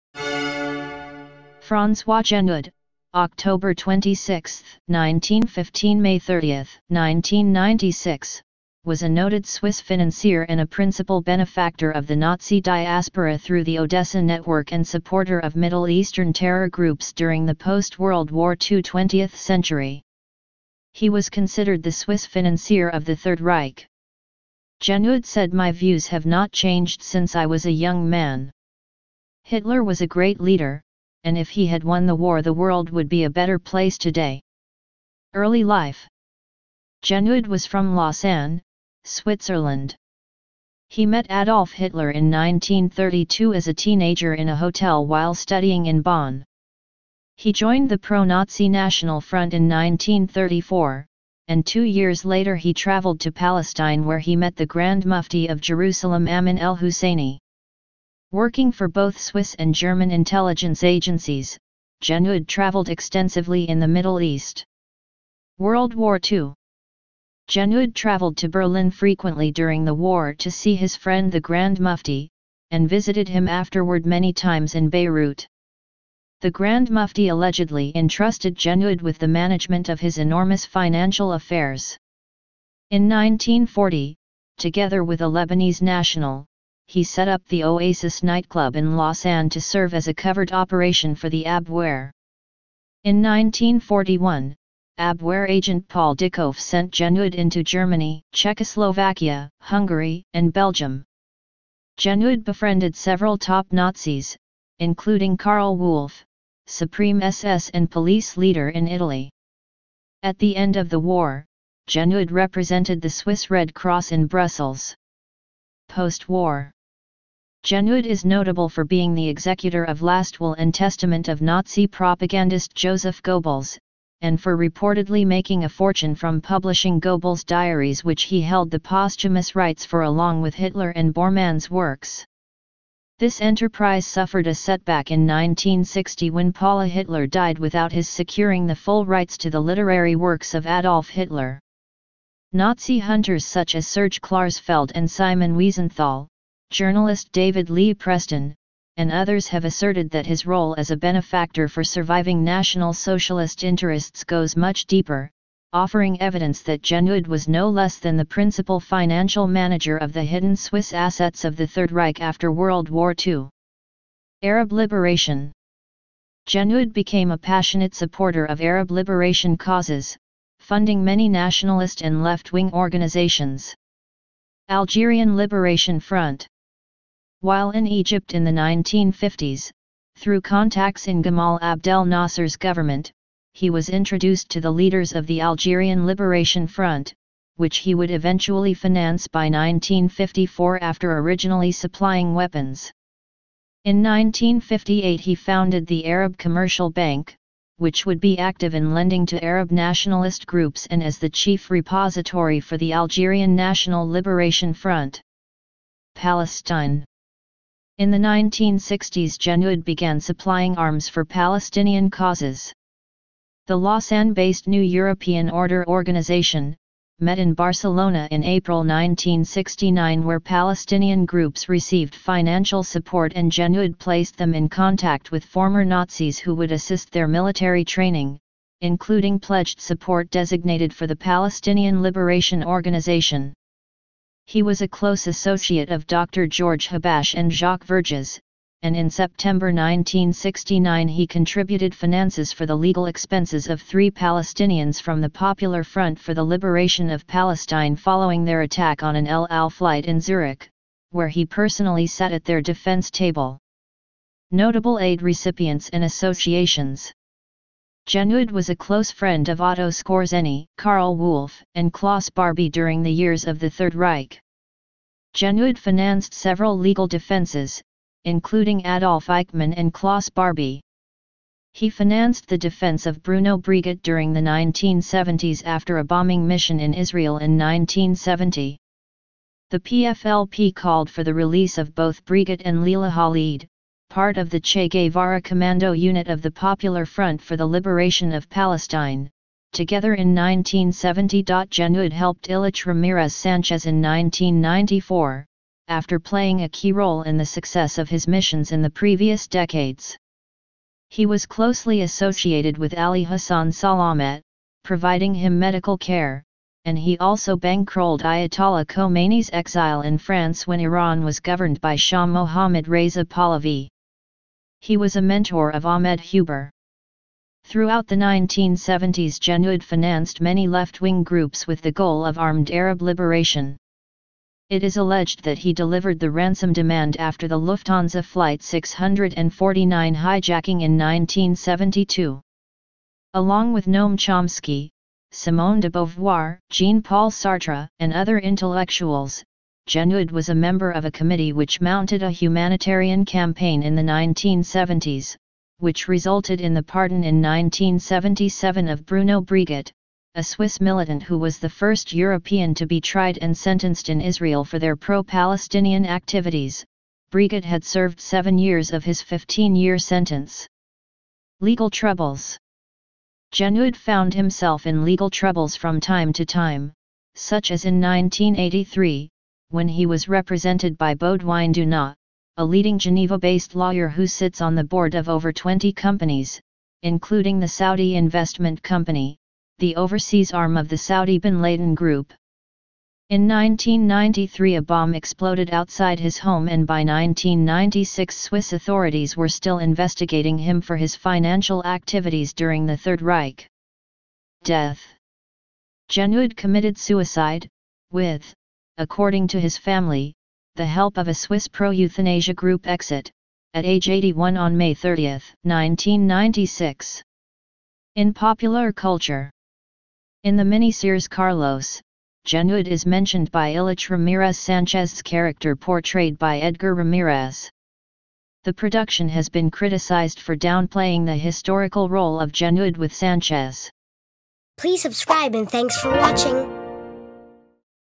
François Genoud - Conspiracies & PseudoScience ✅💡😬💬⁉️
This video is about "François Genoud". This video series is something special. We're fully delving into all things everything and all things about Conspiracy Theories and PseudoScience!

François Genoud (26 October 1915 – 30 May 1996) was a noted Swiss financier and a principal benefactor of the Nazi diaspora through the ODESSA network and supporter of Middle Eastern terror groups during the post-World War II 20th century. He was considered the Swiss financier of the Third Reich.

Here's a couple fun T-shirts

Here's another